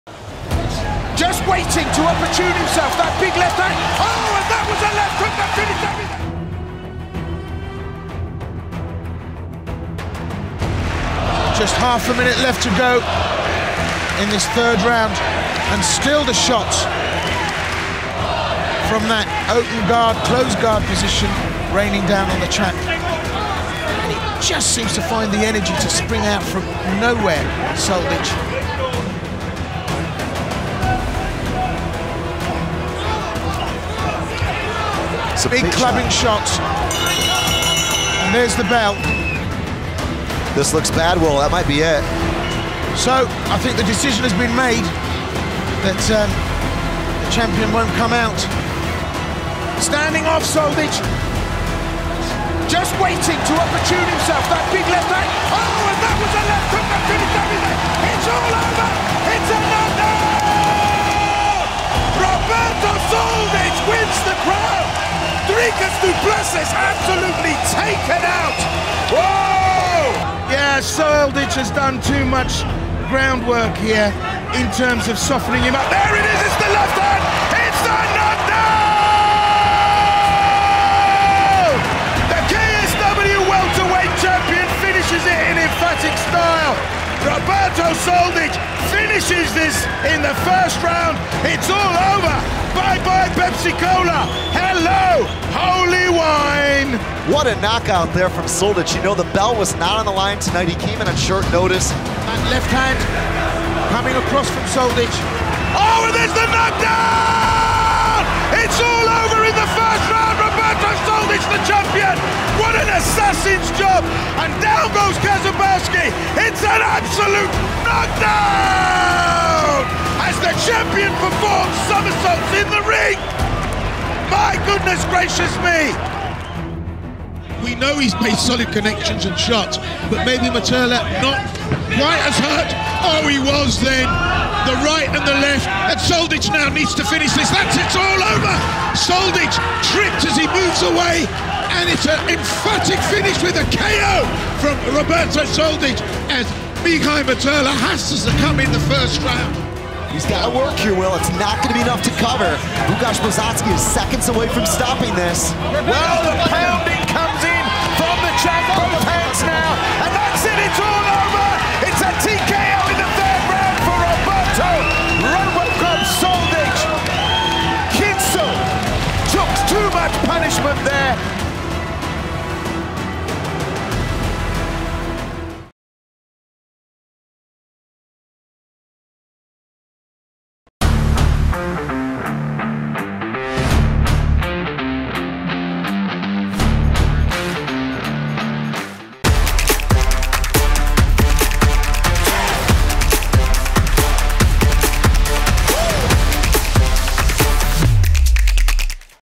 Wszystkie nokauty Roberto Soldicia w KSW | KSW 65
Roberto Soldić wygrał do tej pory w KSW siedem pojedynków i sześciokrotnie nokautował przeciwników. Jeszcze nikt nie zaliczył takiej serii na początku swoich startów w okrągłej klatce i białym ringu KSW. Zobaczcie, jak mistrz kategorii półśredniej kończył zwycięskie boje w KSW.

Gala KSW 65 już 18 grudnia w Arenie Gliwice. Bilety w sprzedaży na eBilet.pl